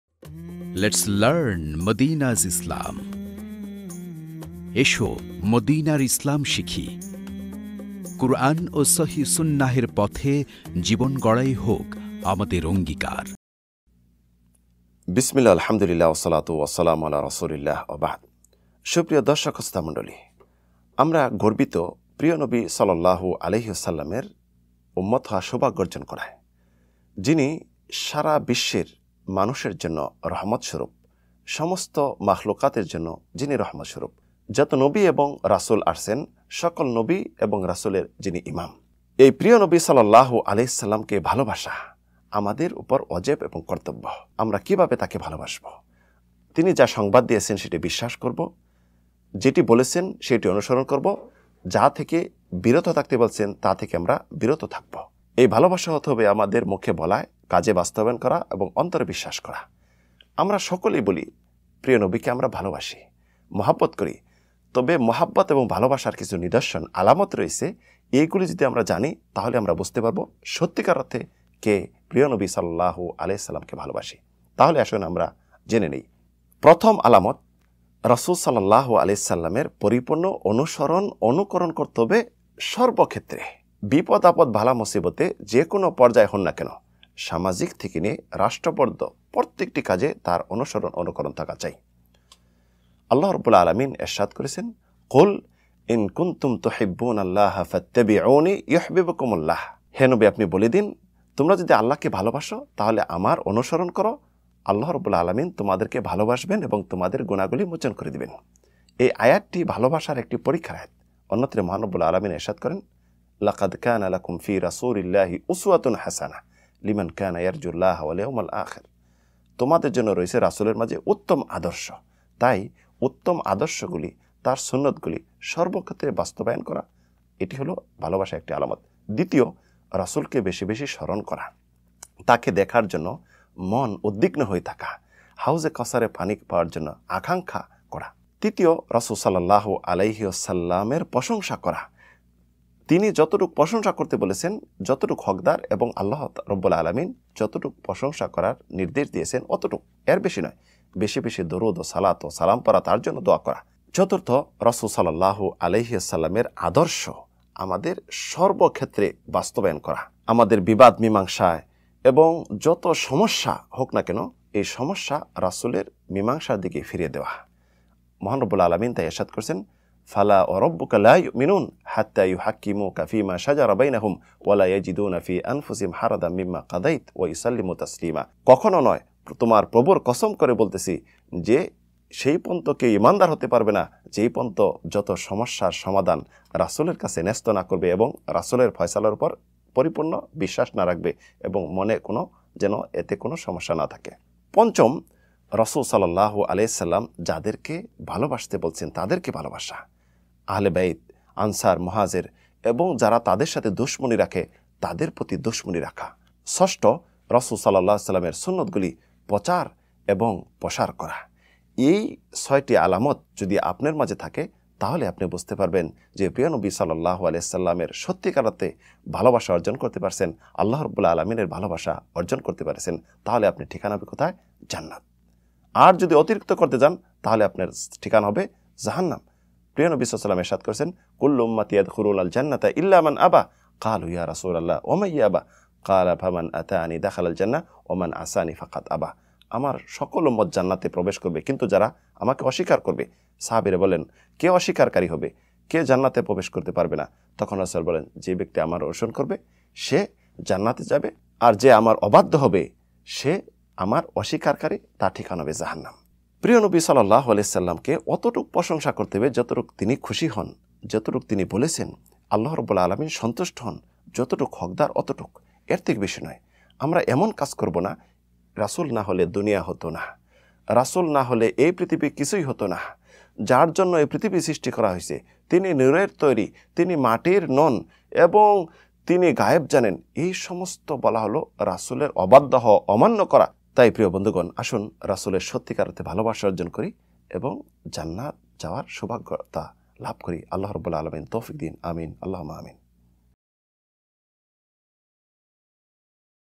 প্রিয় নবী কে ভালোবাসার আলামত ৬টি?(The love of the Prophet)BANGLA LECTURE by Rafiq Bin Lokman Madani
প্রিয় নবী কে ভালোবাসার আলামত ৬টি?(The love of the Prophet)
ভালোবাসার আলামত কি?
রাসূলকে ভালোবাসেন কিভাবে বুঝবেন?
প্রিয় নবীকে ভালবাসা বলতে আমরা কী বুঝি?
রাসূলের ভালোবাসার আলামত! রাসূলের মহাব্বত বলতে কি?
 BANGLA WAZ Bangla lecture by By Rafiq Bin Lokman Madani ,